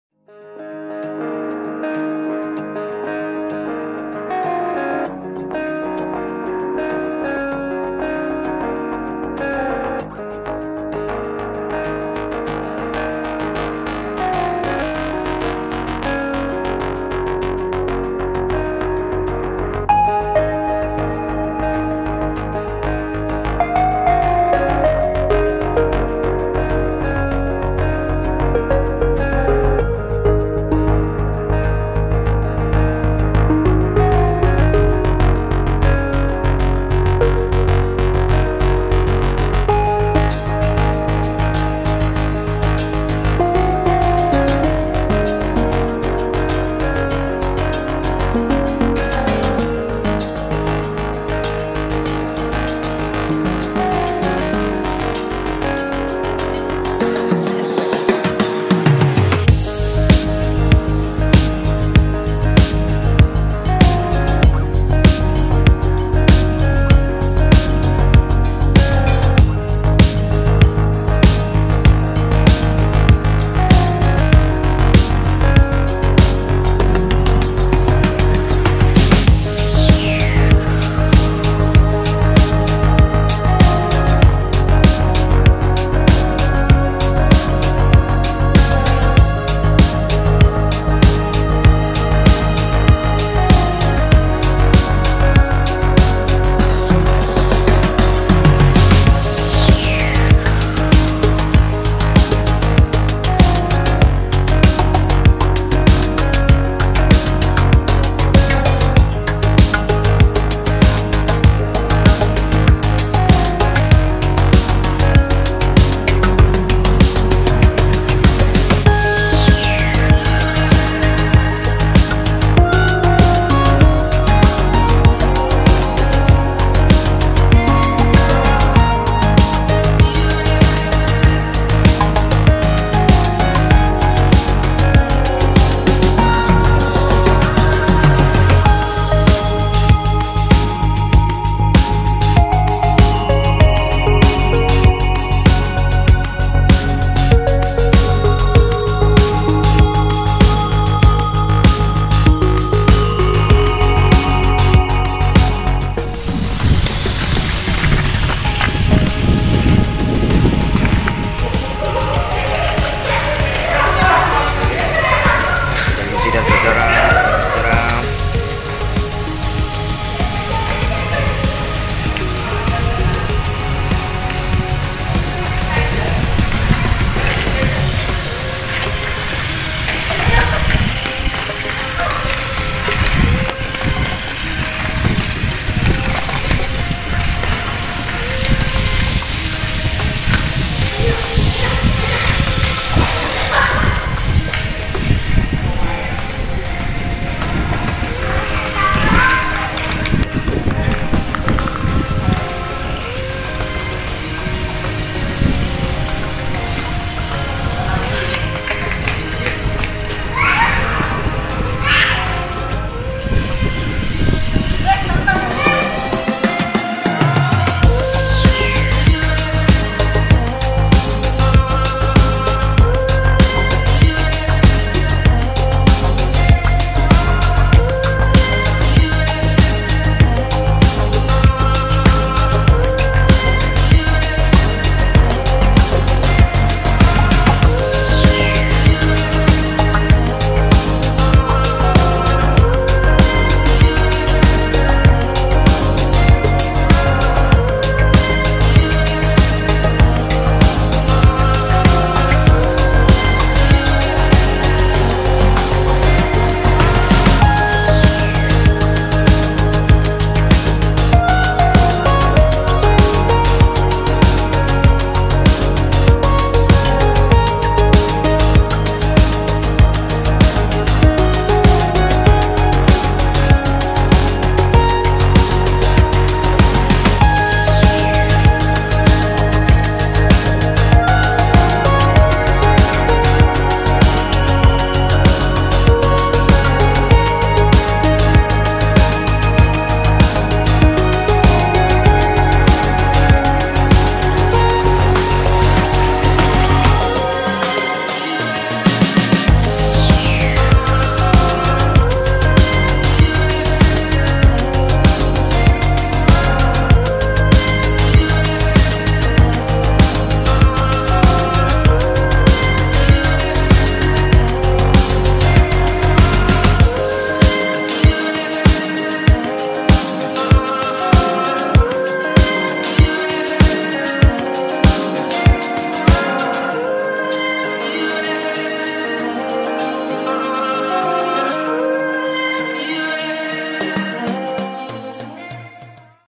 PLAY:ON! Street mini 4wd | Mabar Dengan Anggota Baru
Nah ini dia aktivitas positif dan menyehatkan yaitu bermain mobil mini 4wd sambil berolahraga namanya STREET Mini 4WD. Untuk itu komunitas PlayOn Indonesia yang merupakan komunitas pecinta Tamiya Mini 4WD di kelas Street Run mengajak teman teman untuk bergabung olahraga menyenangkan bersama-sama,
yuk gabung di komunitasnya:

bagi teman teman wilayah kediri dan sekitarnya yang mau bergabung bersama kami silahkan, biasanya kita setiap hari Minggu pagi main di taman Brantas, Hutan Joyoboyo kalau hari Selasa di Simpang Lima Gumul di area pasar tugu